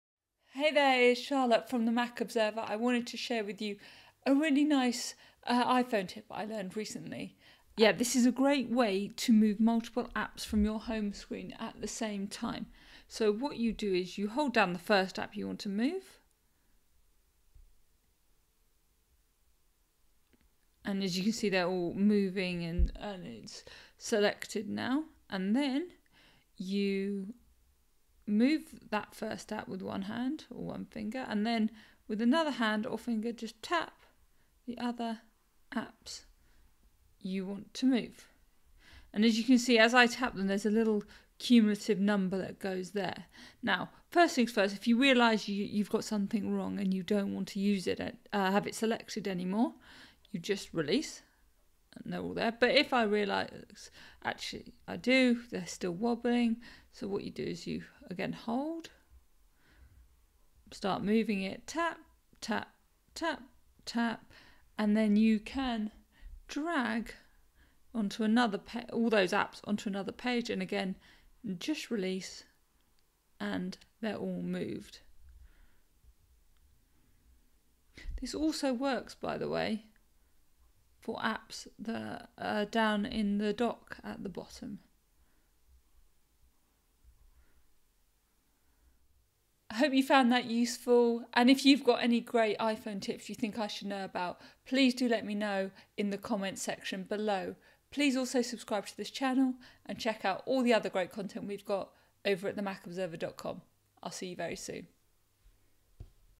How to select and move multiple iPhone Apps at the same time.
If you need to move a number of iPhone apps around on your Home Screen it can be time-consuming to do it one-by-one. However, there is a way to select and move multiple iPhone Apps at the same time.

Hold down the first app you want to move. The apps on your Home Screen will start 'wobbling'.
Begin moving that first app.
While still holding it down, use another finger to tap the other apps you wish to move - a blue number indicating the number of apps selected will appear.
When you have selected everything you want to shift, keep holding down and move the selection to where you want them to go on the Home Screen.

Using this method means you can move a number of items onto a separate page or into a folder in one go. It also works with those apps in your dock. If you realize that you've selected the wrong item, simply release and everything is unselected again.